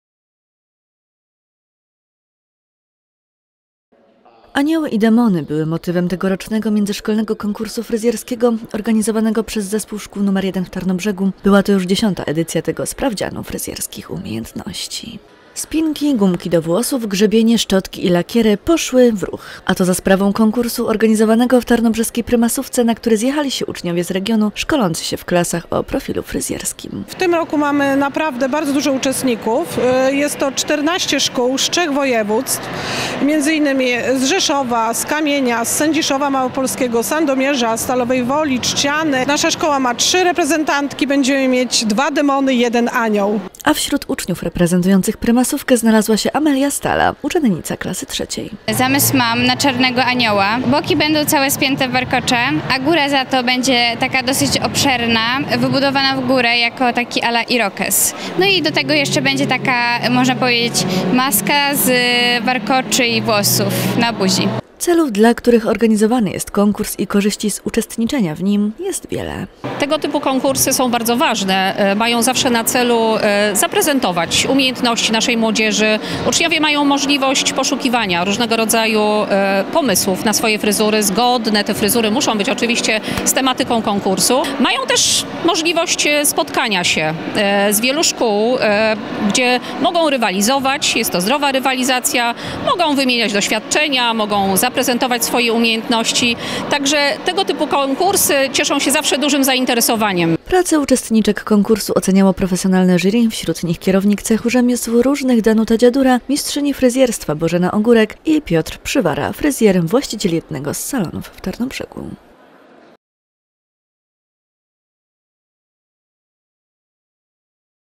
2023.11.16 - FRYZJERSKI FACH W RĘKACH
Anioły i Demony były motywem tegorocznego Międzyszkolnego Konkursu Fryzjerskiego, organizowanego przez Zespół Szkół nr 1 w Tarnobrzegu. Była to już 10 edycja tego sprawdzianu fryzjerskich umiejętności.